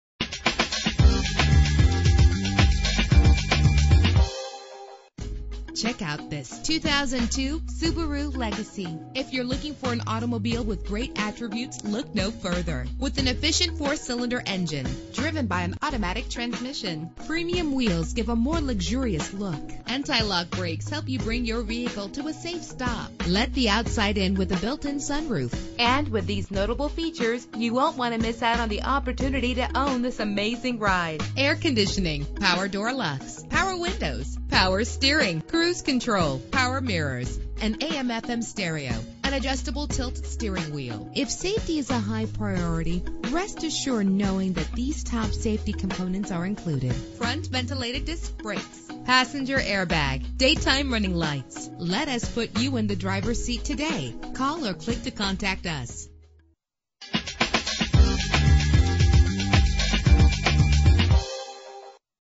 2002 Subaru Legacy - Philadelphia PA
Year: 2002
Make: Subaru
Model: Legacy
Trim: GT
Engine: 2.5 liter H4 16 valve
Transmission: Automatic
Color: Red
Mileage: 58663
Address:
6723 Essington Ave.